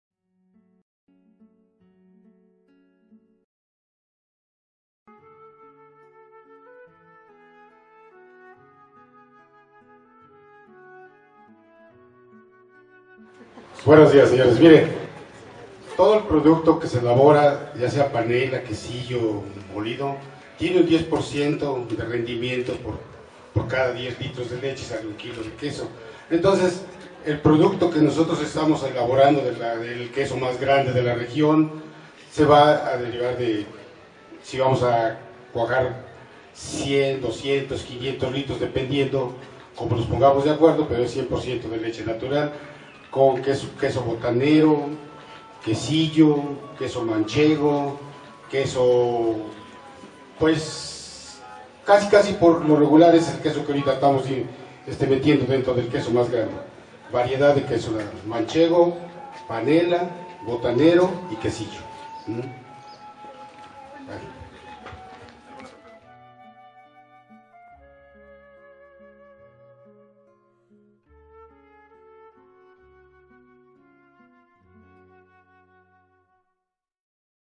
José Porquillo
José Porquillo, productor de quesos, explica sobre el queso más grande de la región, a partir el domingo 2 de agosto a las 15 horas.